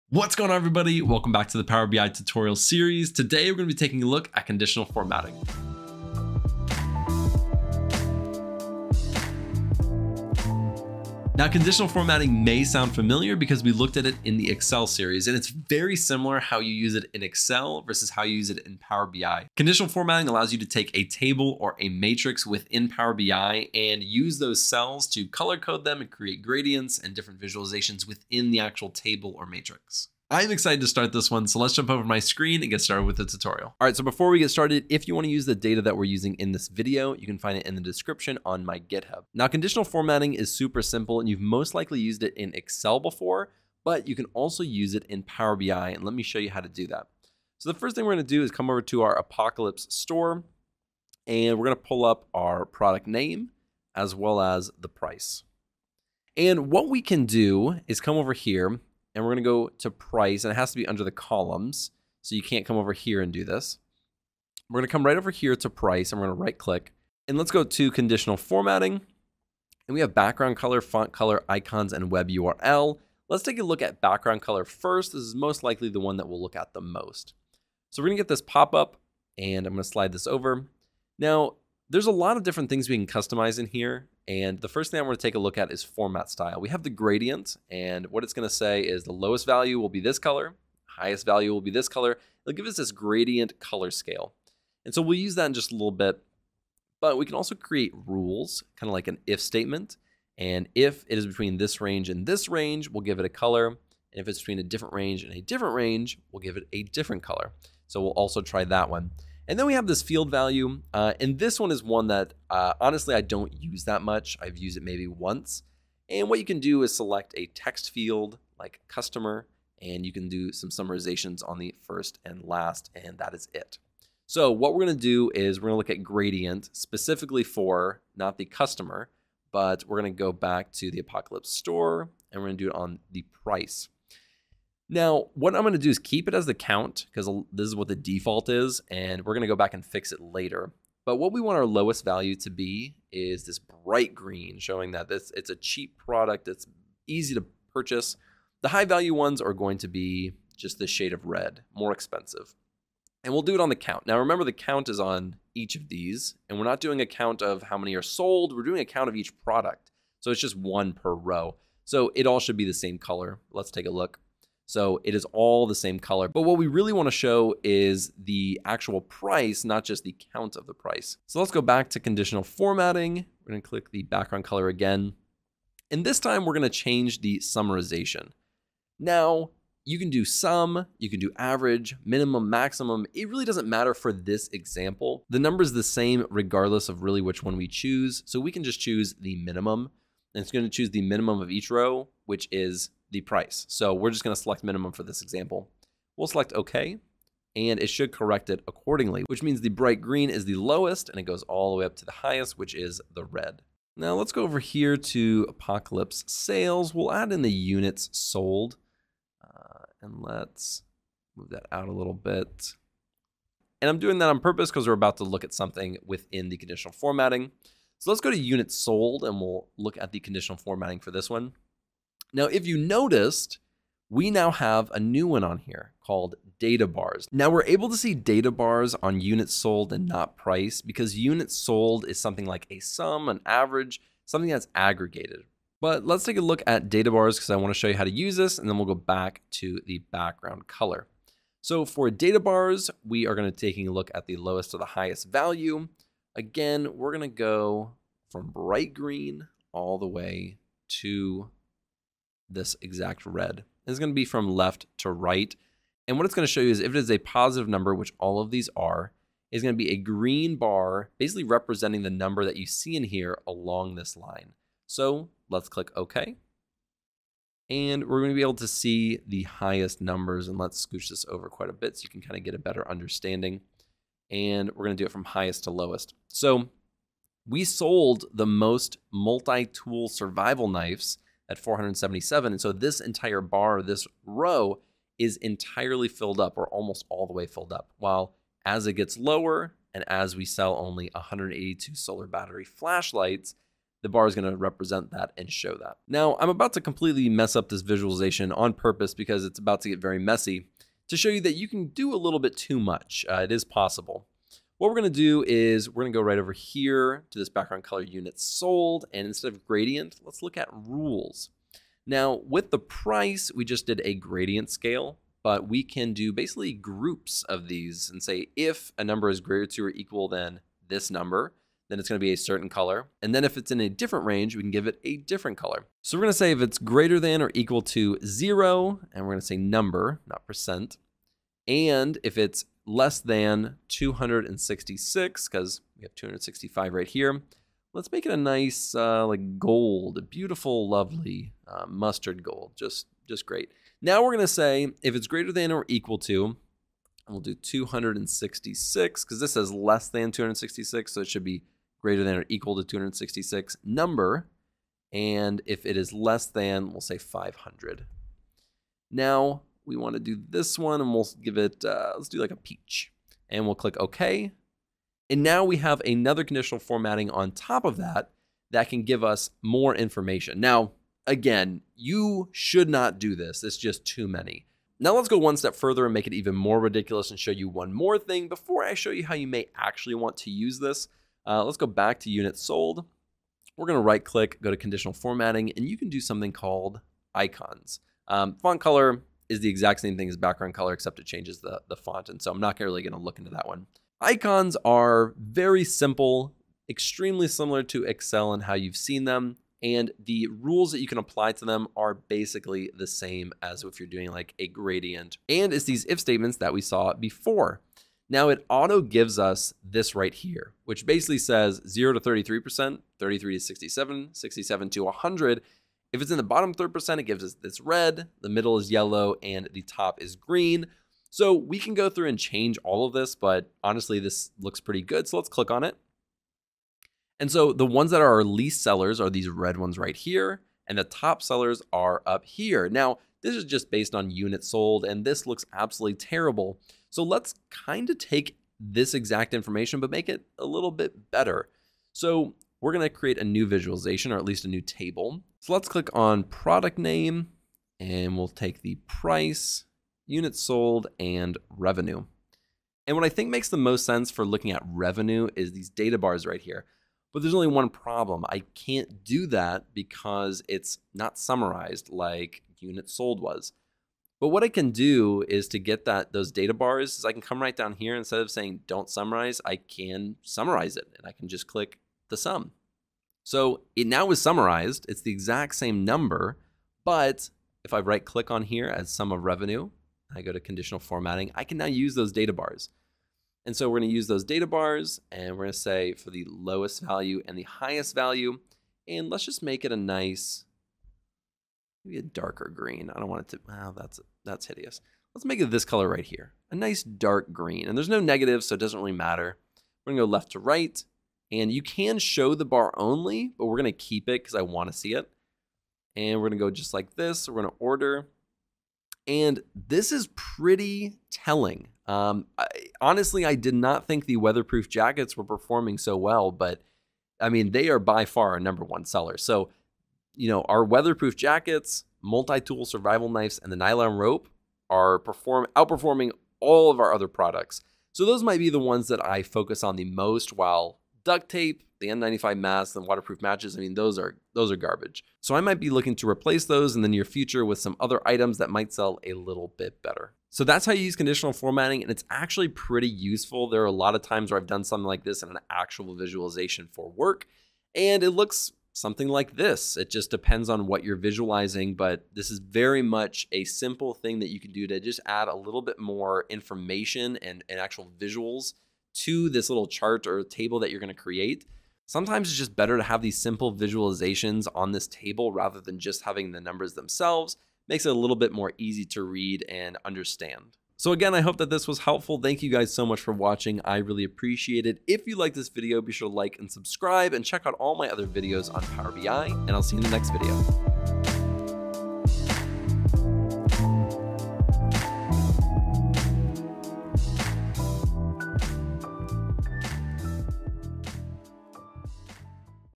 How to use Conditional Formatting in Power BI | Microsoft Power BI for Beginners
In this series we will be walking through the basics of Microsoft Power BI. In this video we will be walking through how to use Conditional Formatting in Power BI.